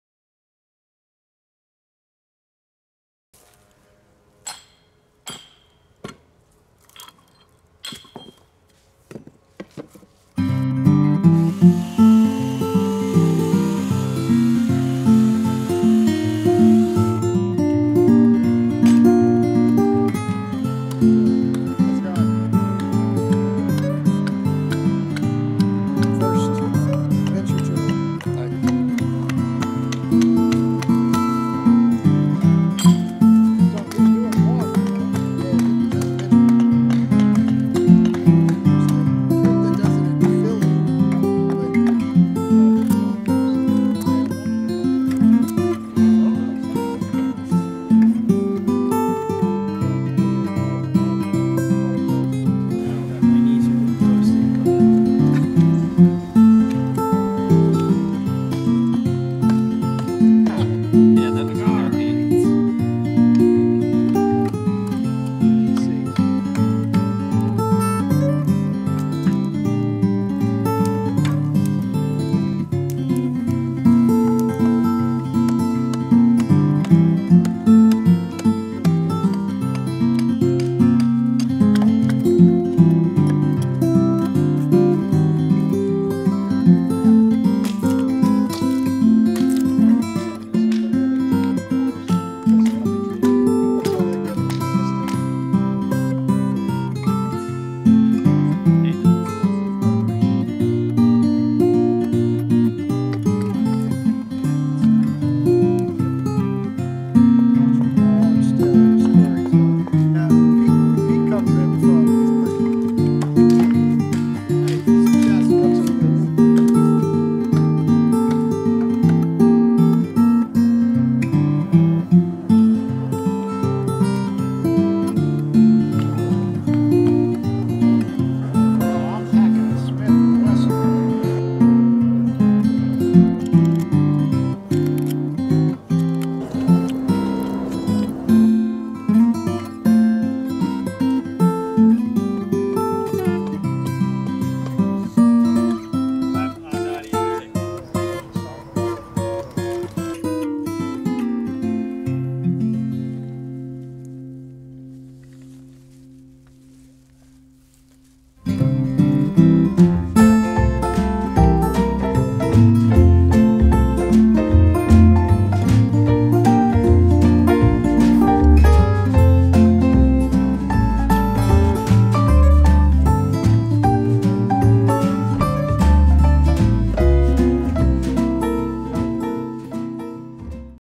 DC Spoon Carvers
Shortened version of the DC Spoon Carver meetup last year, November, 2019.
Nate Bour, Damien Ossi, Don Nalezyty